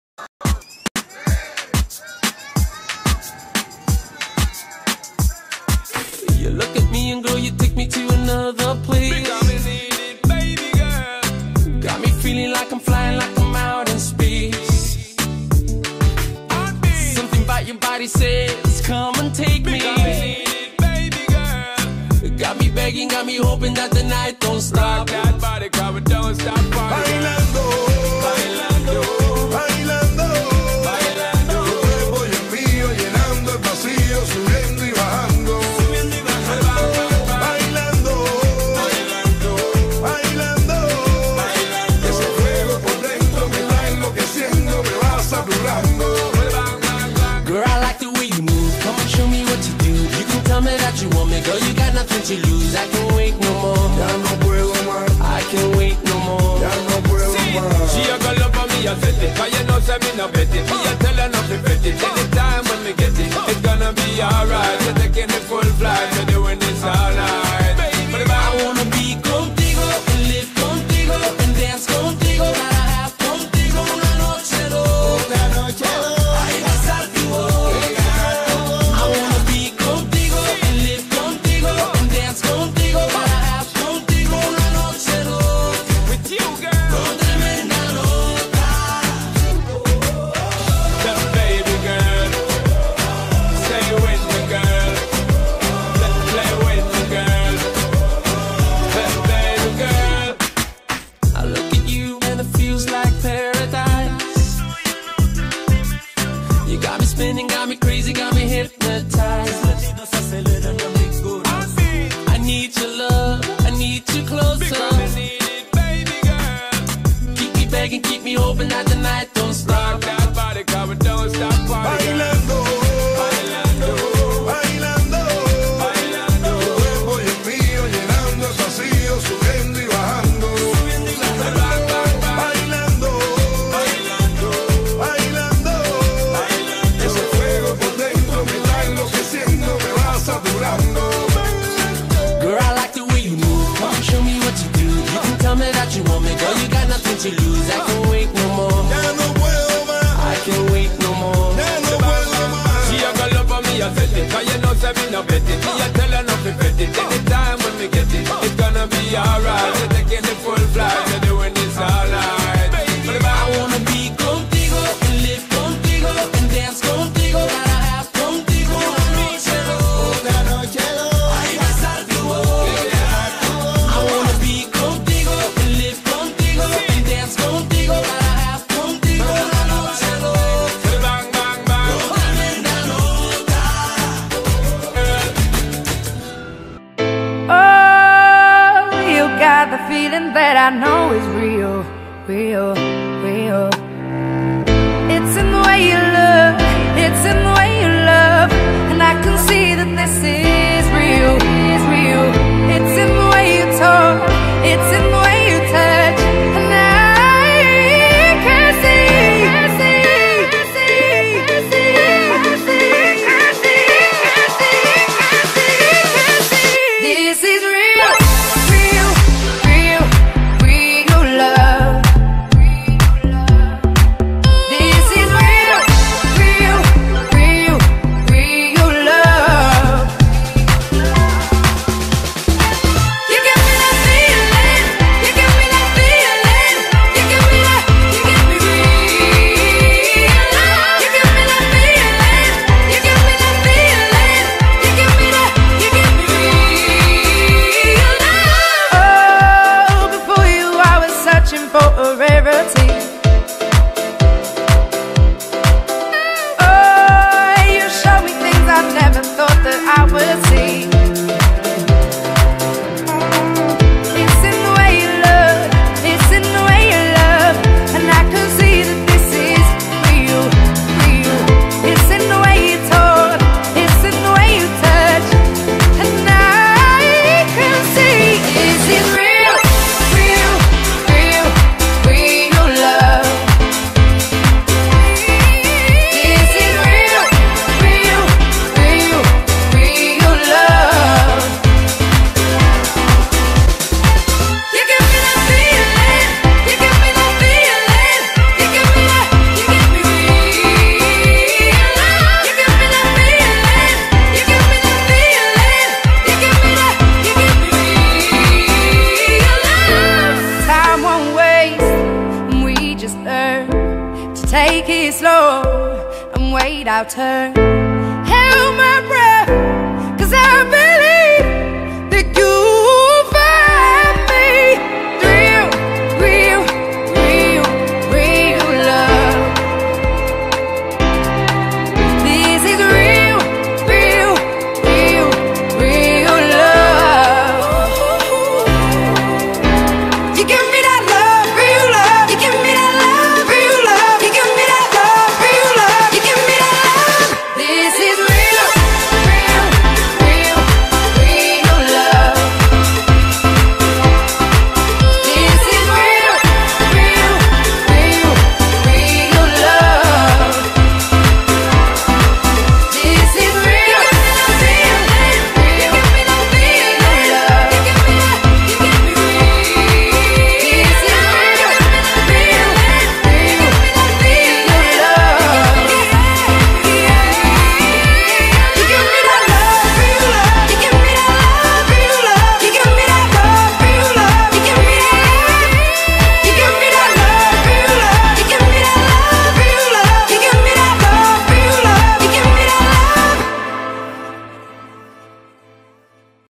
Luciano 'Lucho' Vázquez - Al Shahaniya 2014/15
Name: Luciano Claudio Vázquez
Nationality: Argentine
DoB: 06/03/1985
Position: Striker
Height/Weight: 181cm / 81kg